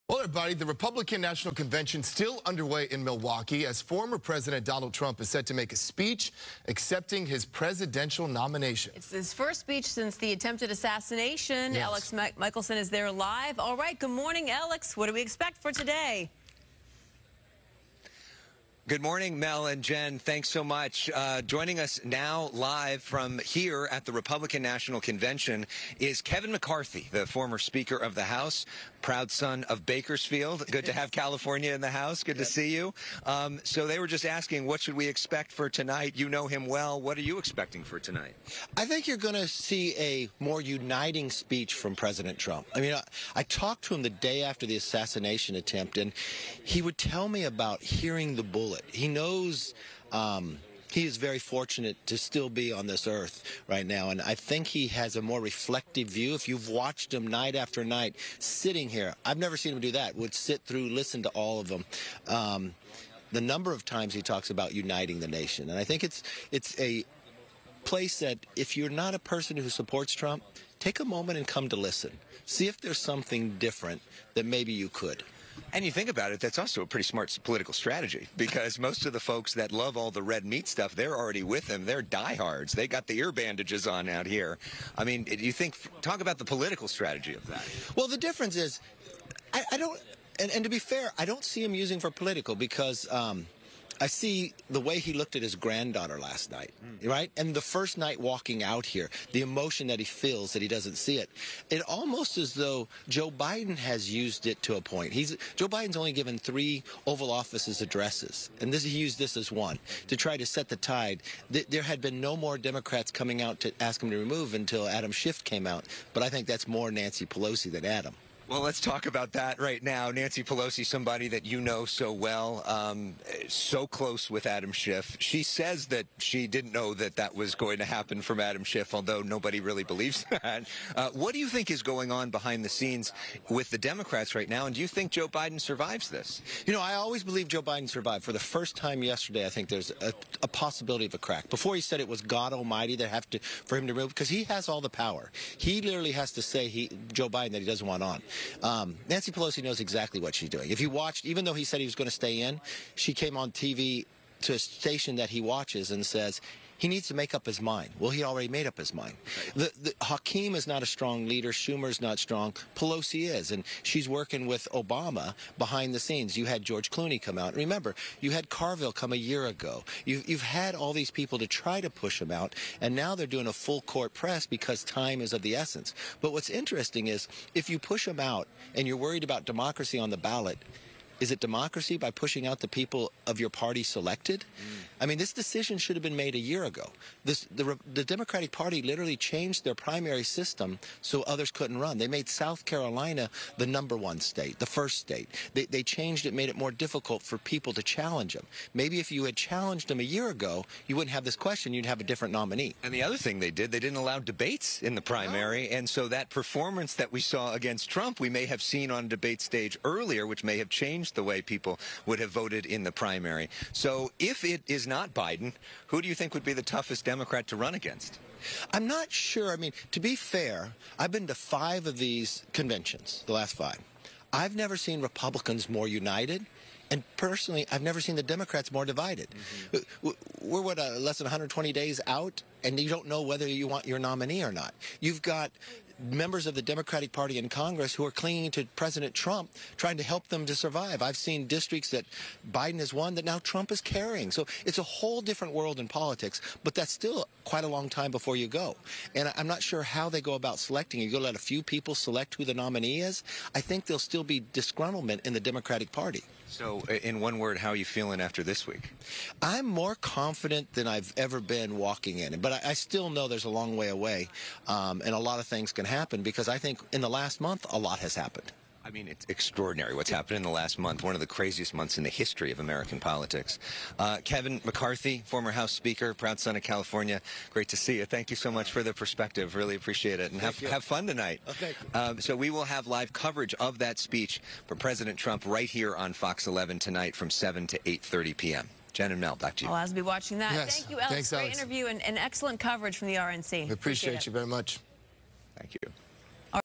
RNC Day 4: What to expect as Trump takes stage
Elex Michaelson speaks with former House Speaker Kevin McCarthy about what to expect for tonight.